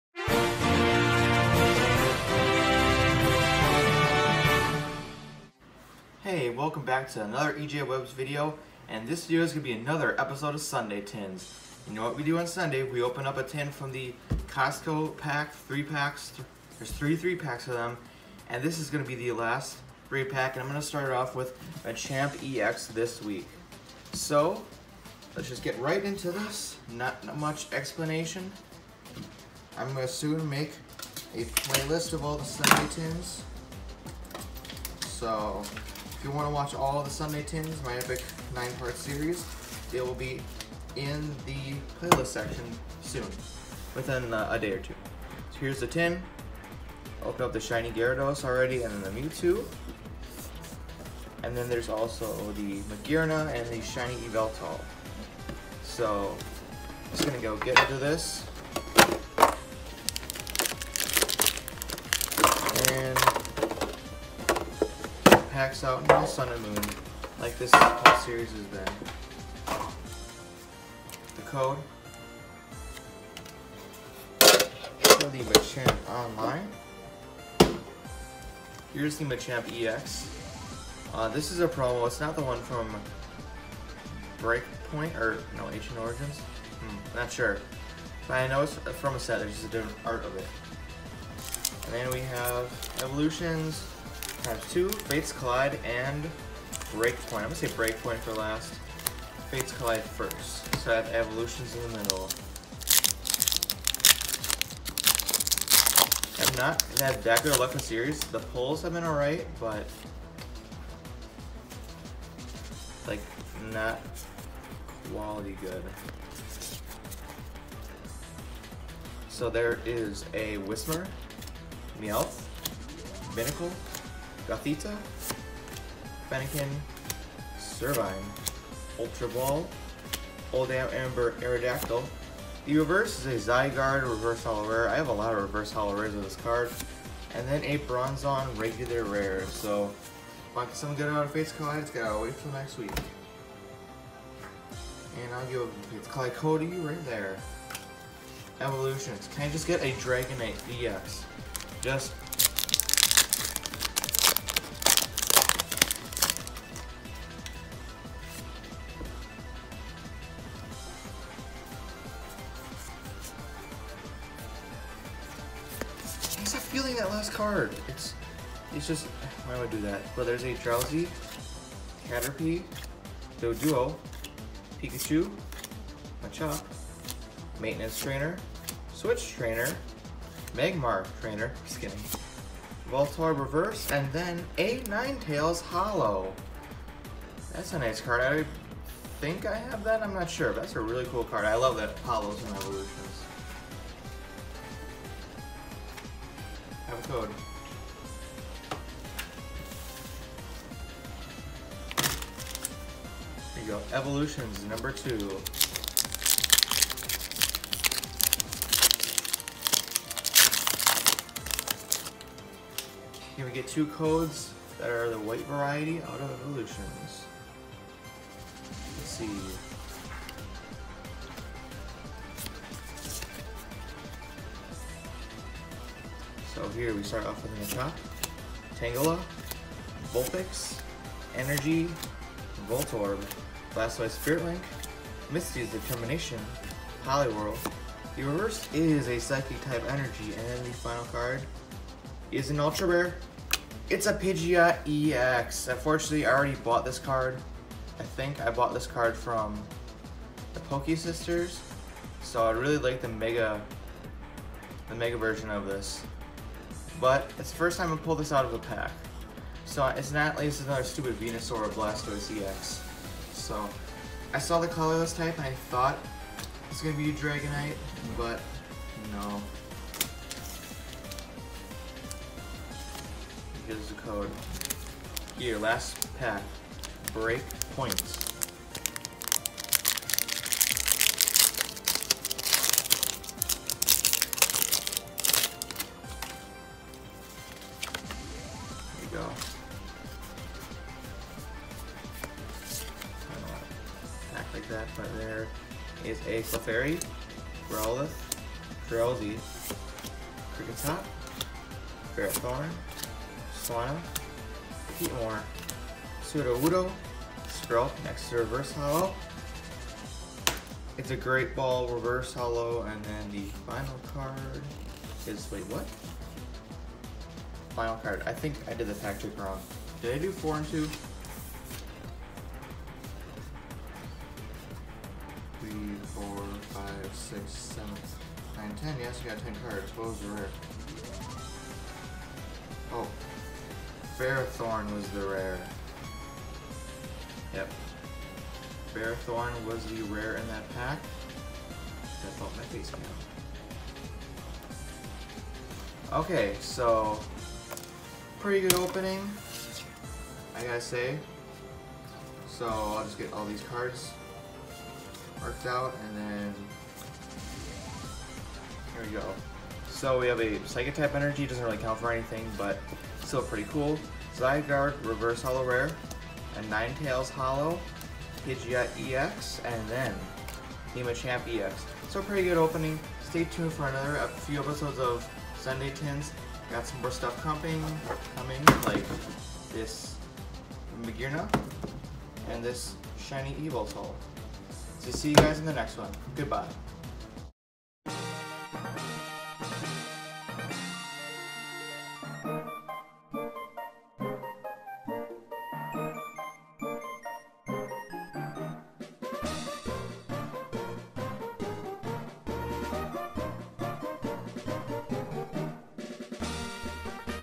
Sunday tins 7: Machamp ex tin
Here I open another tin in my 9 part series.